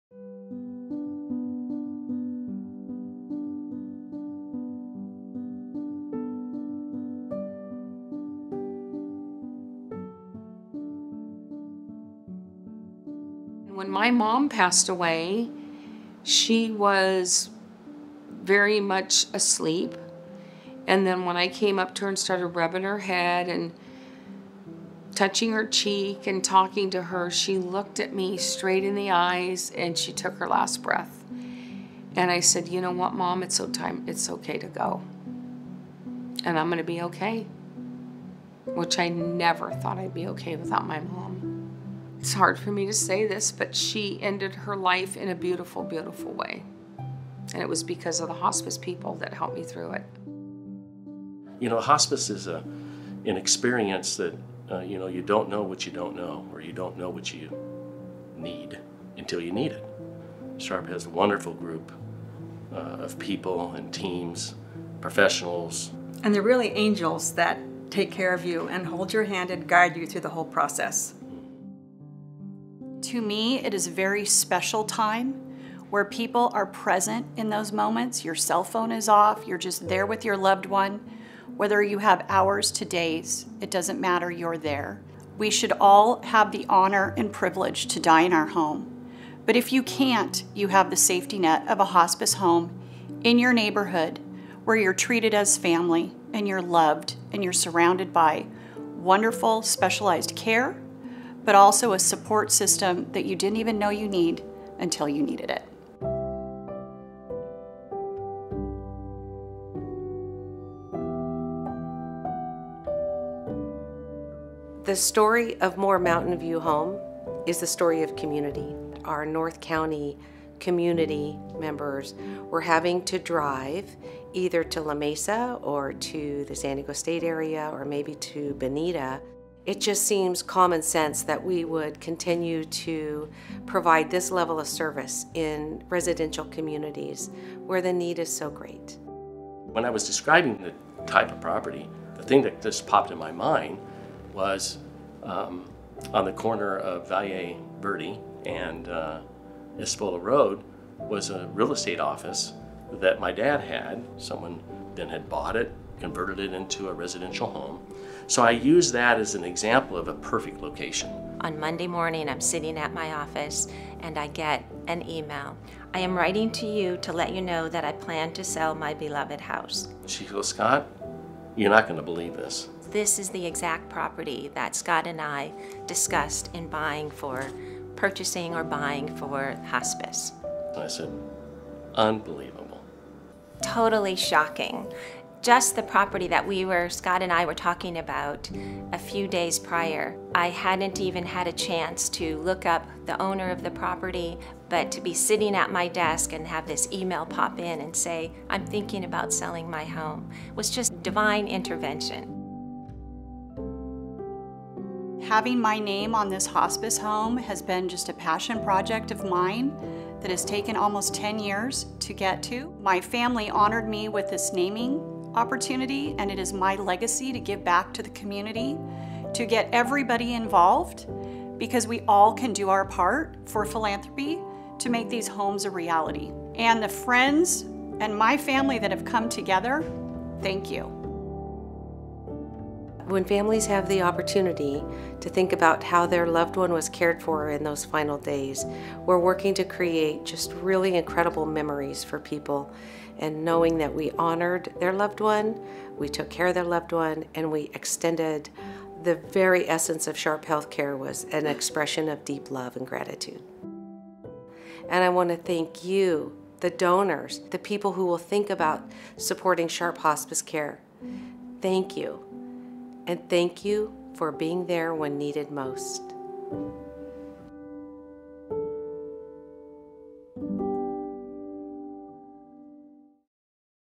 Bringing Compassionate Hospice Care to North County
At Sharp HospiceCare, hospice is more than end-of-life support. It's about comfort, compassion and giving every patient the best possible quality of life. That’s why we offer unique, home-like residences devoted solely to patients and their families — a warm environment, paired with skill and compassionate care, when it counts the most.

Sharp HospiceCare’s newest home, Moore MountainView Hospice Home, is set to open in Poway in 2025 and is a vital expansion, addressing the growing need for compassionate end-of-life care in North County. Moore MountainView, named after a generous gift from Lori Moore, RN, and the Cushman Foundation, is made possible due to the generosity of many.

To learn more about Moore MountainView and to show your support,